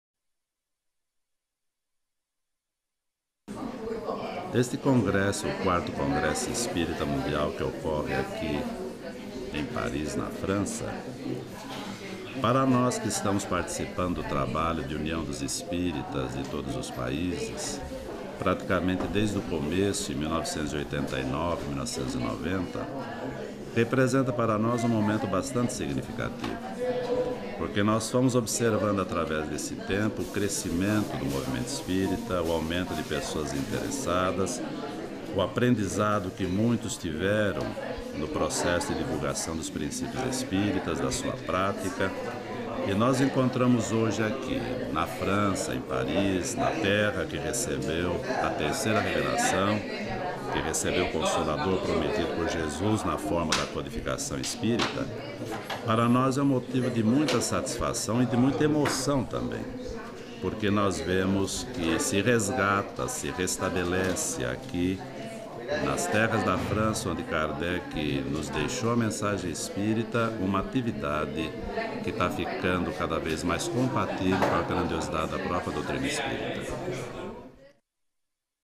A SATISFAÇÃO DE SERVIR A UMA CAUSA NOBRE
Em Paris, durante o Congresso Espírita Mundial, em homenagem ao bi-centenário de nascimento de Allan Kardec, Nestor Masotti expressa sua satisfação pela homenagem ao amável mestre que, desde sua adolescência, ele ajudou a divulgar, primeiramente na juventude espírita. Depois, como dirigente de Centro Espírita e presidente da USE - União das Sociedades Espíritas do Estados de São Paulo. Mais tarde como vice-presidente da FEB e depois presidente e Sec. Geral do CEI.